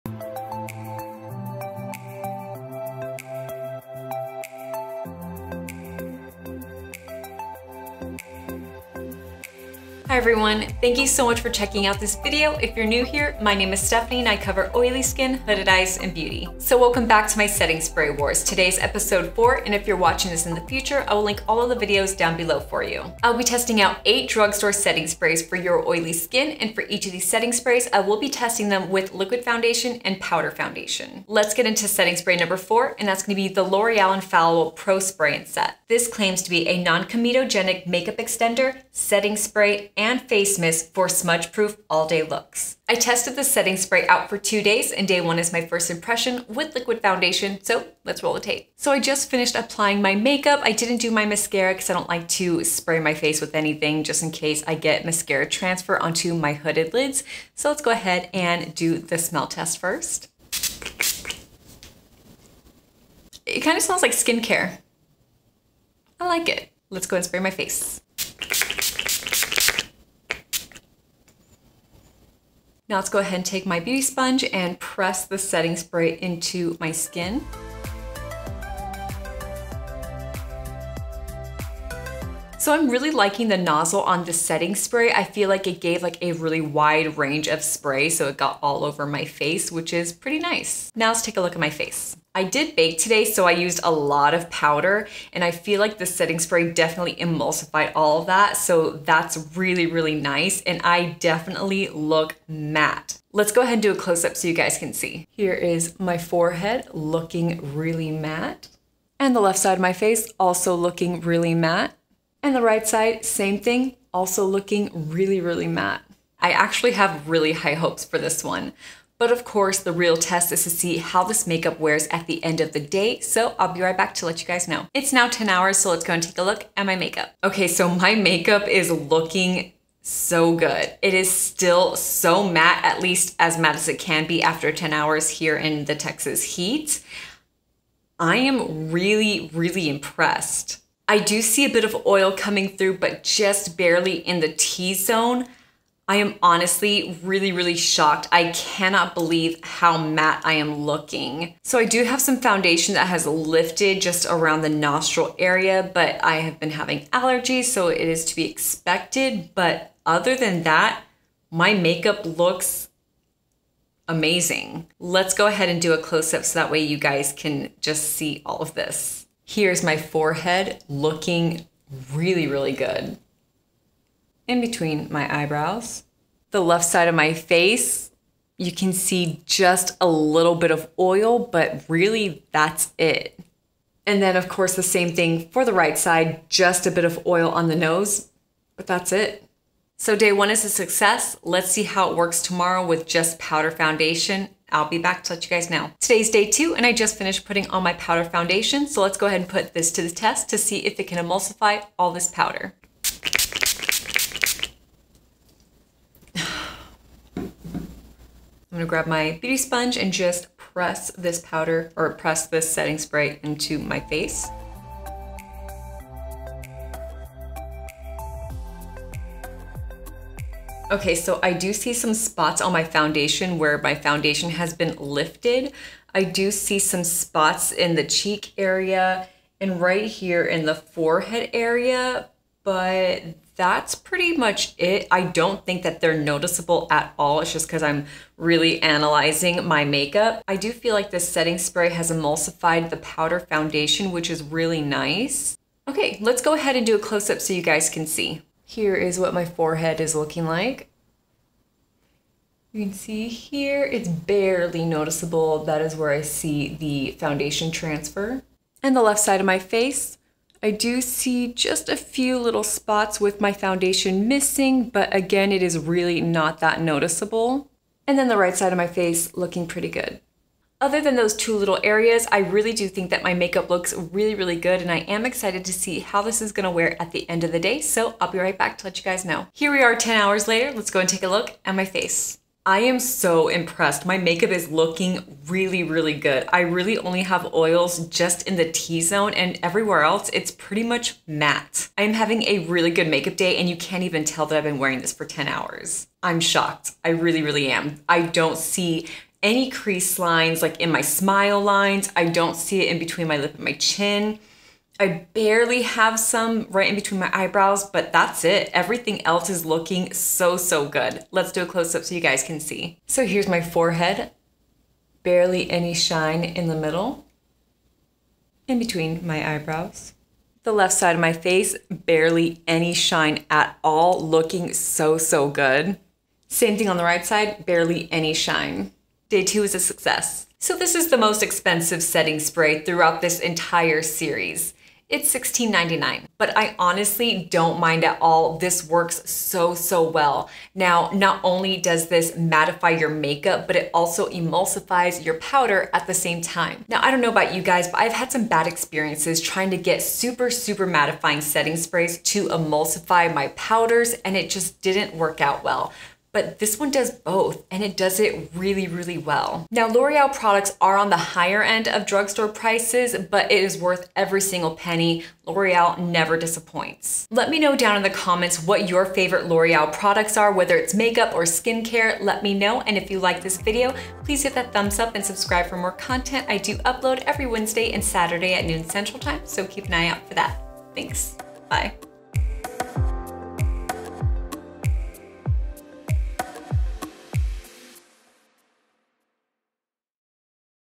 L’Oreal Infallible Pro-Spray & Set Makeup Extender-Setting Spray Wars - Oily Skin(Drugstore Edition)
In today’s video, I will be testing out the L’Oreal Infallible Pro-Spray & Set Makeup Extender for your oily skin.

Face:
Charlotte Tilbury Magic Serum Crystal Elixir
L’Oreal Infallible Matte-Lock Mattifying Primer
Wet n Wild Photofocus Foundation - Soft Beige
Maybelline Instant Age Rewind Eraser Multi-Use Concealer - 120 Light
Wet n Wild Photofocus Loose Setting Powder - Translucent
Nabla Skin Bronzing Sunkissed Effect Bronzing Powder - Ambra
Nabla Skin Glazing Glass Skin Finish Glow Powder - Truth
Nabla Skin Glazing Glass Skin Finish Glow Powder - Ozone
L’Oreal Infallible Pro-Spray & Set Makeup Extender

Eyebrows:
Benefit Cosmetics Goof Proof Brow Pencil - 3
Benefit Cosmetics Precisely, My Brow Pencil - 3
Anastasia Beverly Hills Brow Freeze Brow Styling Wax

Eyes:
Rare Beauty Always An Optimist Weightless Eye Primer
Rare Beauty Stay Vulnerable Liquid Eyeshadow - Nearly Neutral
Colourpop Cosmetics Super Shock Shadow - Good Boy!
KVD Tattoo Liner - Trooper Black
Too Faced Better Than Sex Mascara

Lips:
Maybelline Vivid Hot Lacquer - 62 Charmer

I upload new videos here every Wednesday & Saturday at 12 pm CST!